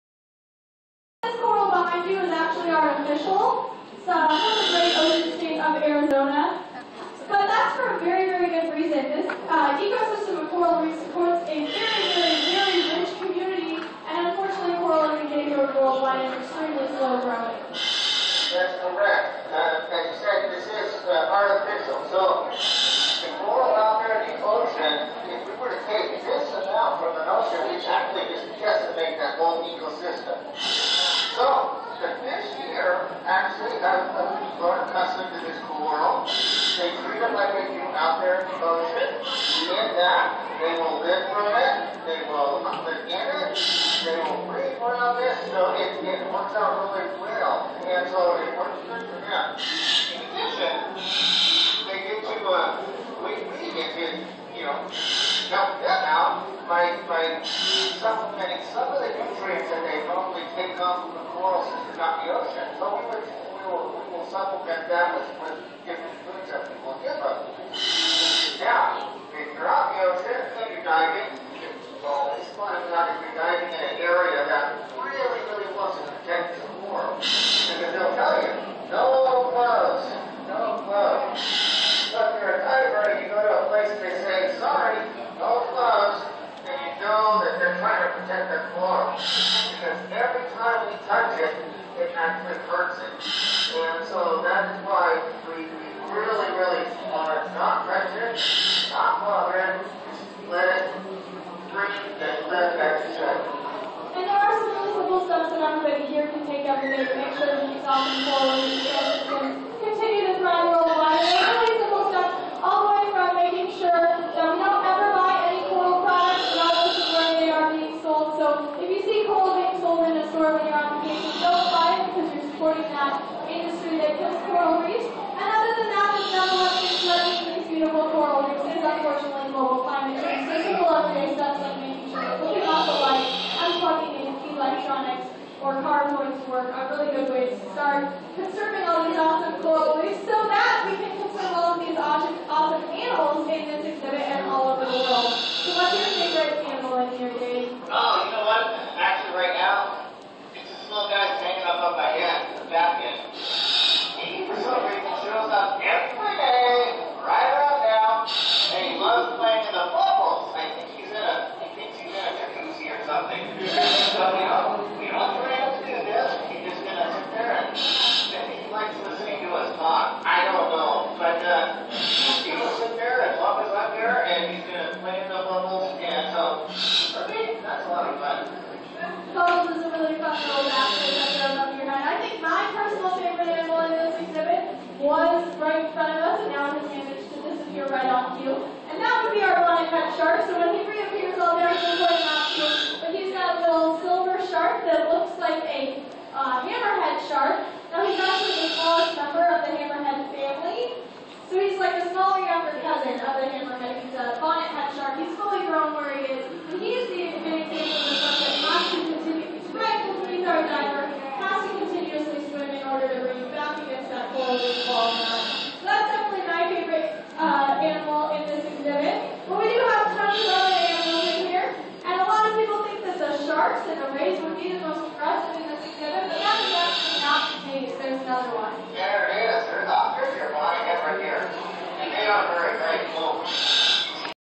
Leafy Sea Dragon Seahorse
This was taken at the Aquarium Of The Pacific, Long Beach, California